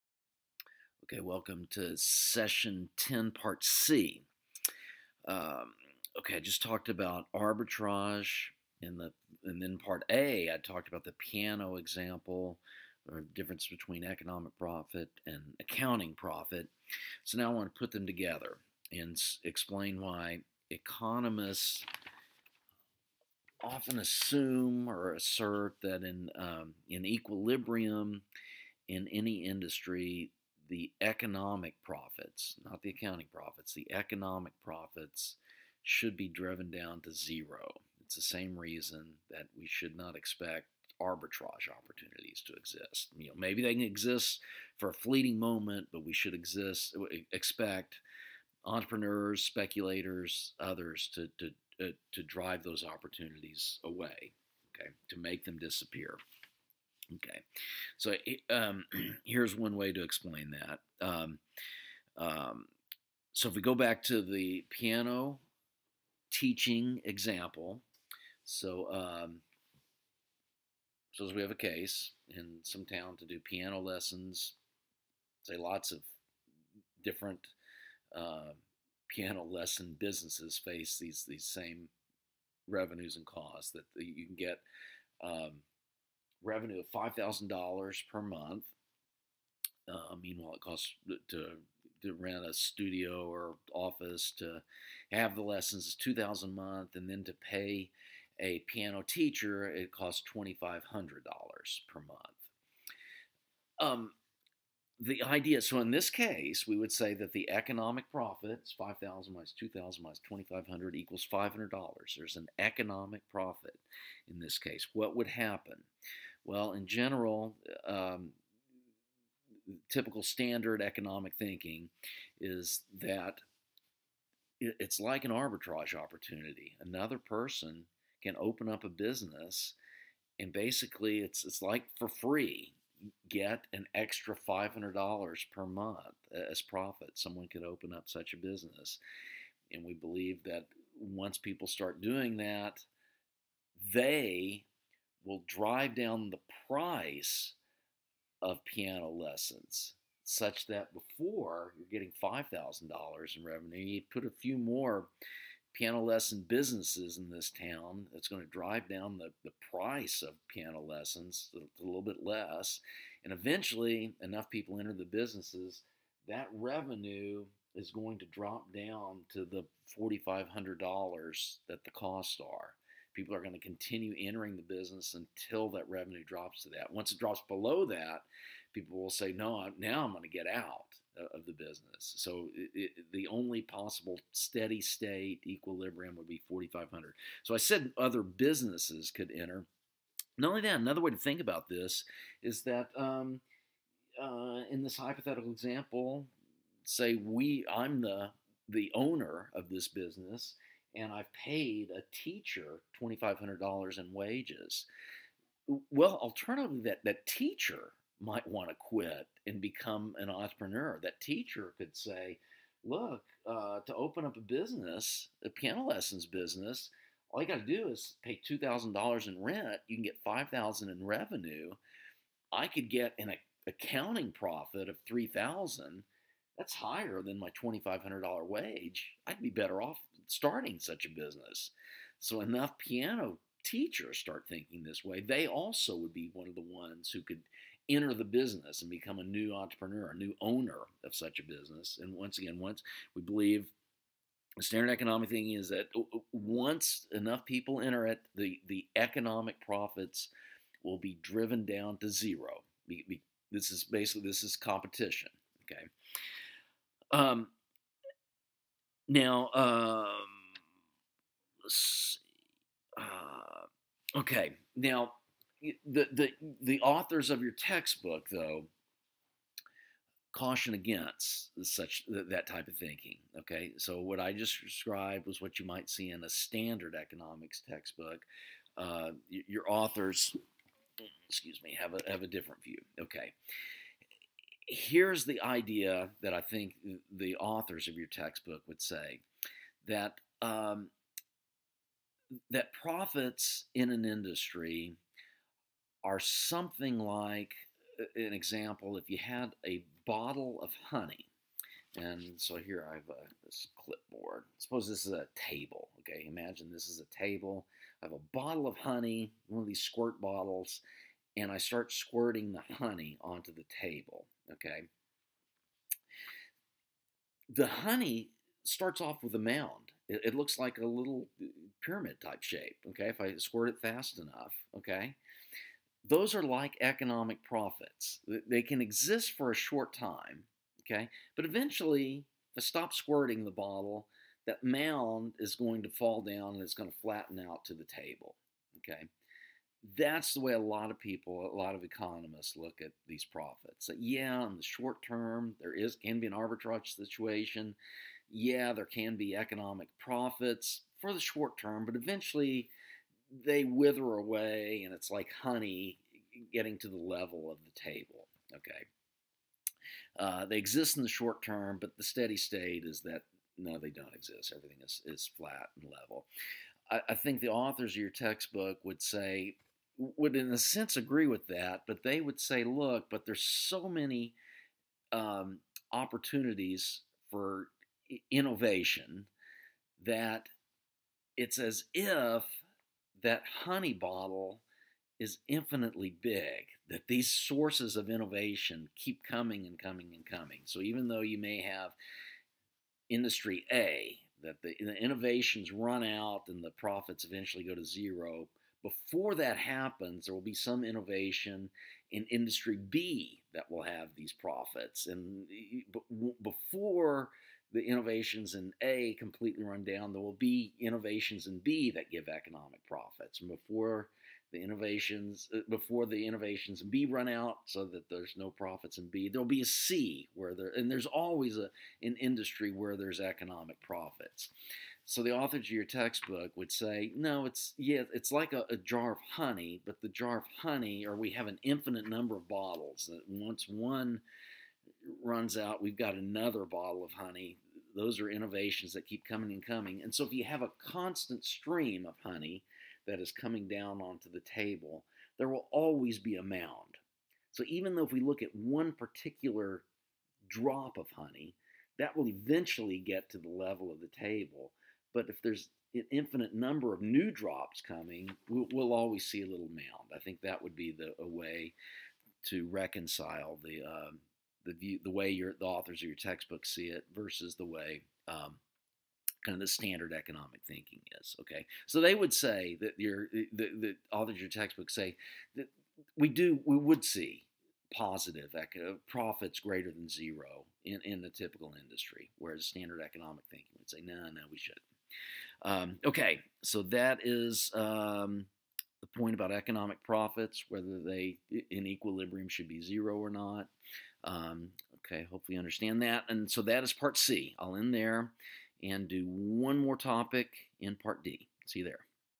T. Groseclose: Econ 100: Lecture 10c
Standard economic thought:  In equilibrium economic profits are zero.  Economics joke:  "No, there can't be a $20 bill on the sidewalk; in equilibrium someone would have already picked it up."  Even innovations only give a temporary economic profit.  By contrast, Austrian school of economics says that economic profits CAN exist, even in the steady state.  Analogy: pouring honey onto a countertop.